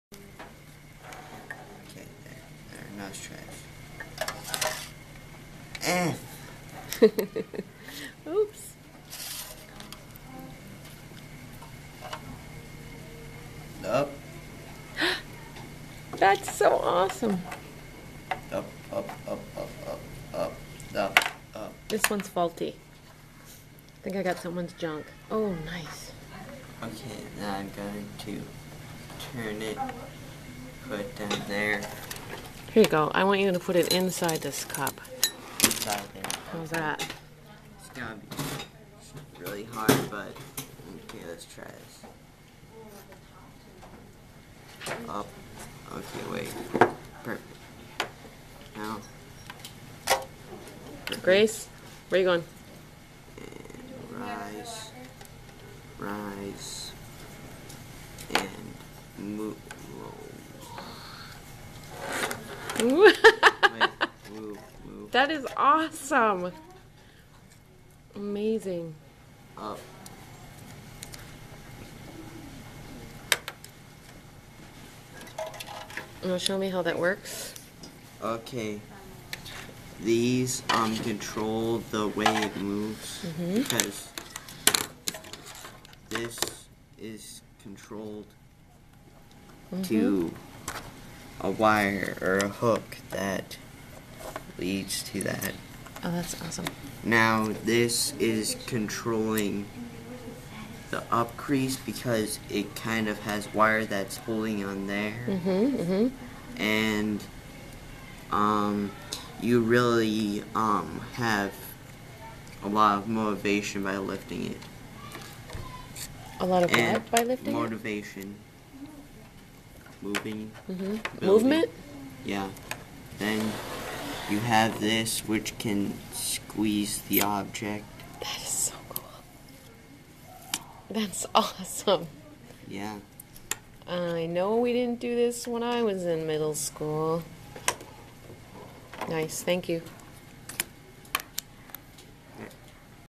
Middle School Robotic Arm Project
6th graders worked in small groups (with a talented Design and Modeling teacher) to build these.  How cool is that?!